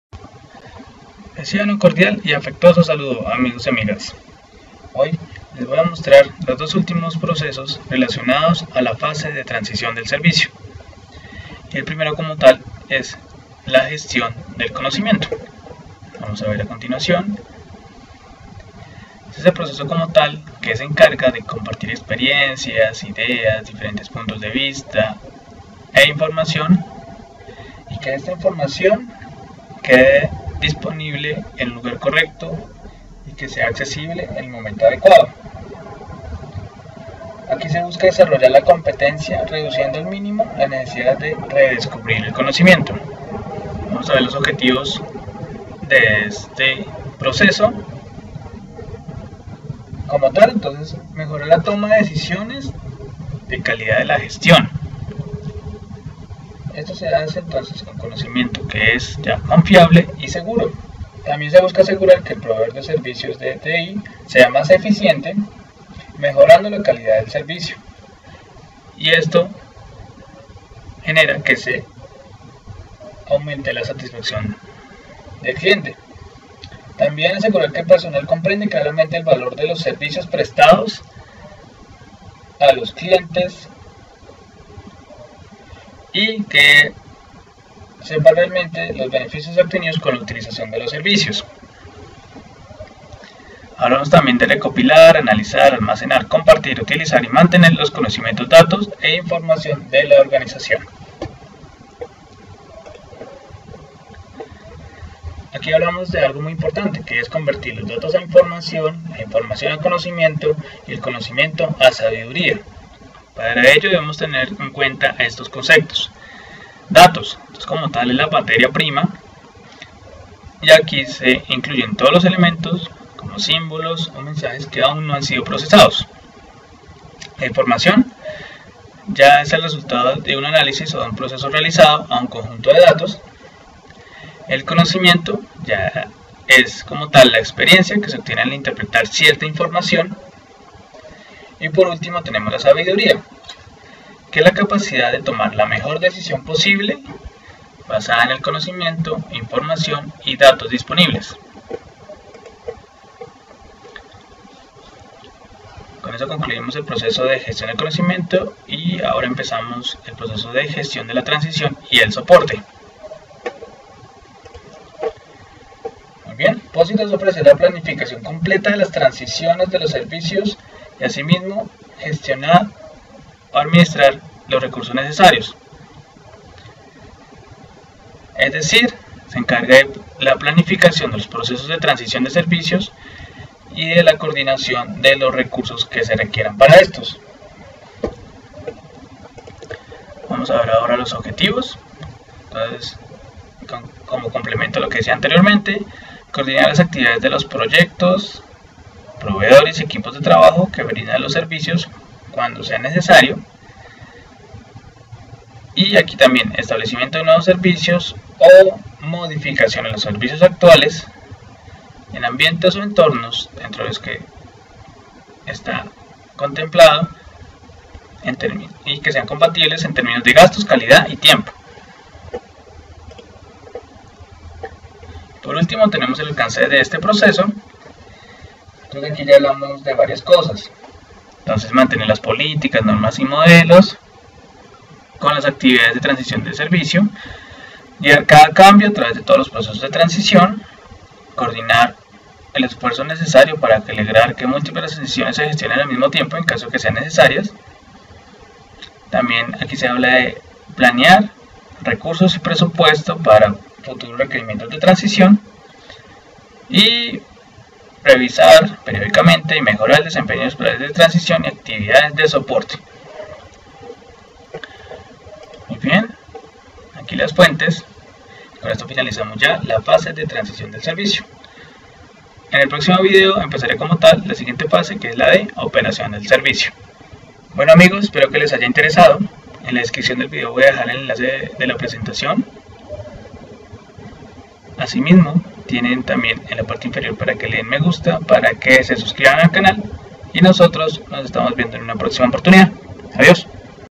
Transición del servicio (parte 5)
Vídeo ilustrativo sobre el proceso de gestión del conocimiento y
gestión de la transición y el soporte
 (Quinta parte de la fase de transición del servicio).
Fuentes:
Extractos sacados del libro Gestión de Servicios de Tecnología de la
Información - Fundamentos de ITIL
Ismael A. Ramírez
Érika Zuluaga
La presentación la pueden encontrar en este siguiente enlace: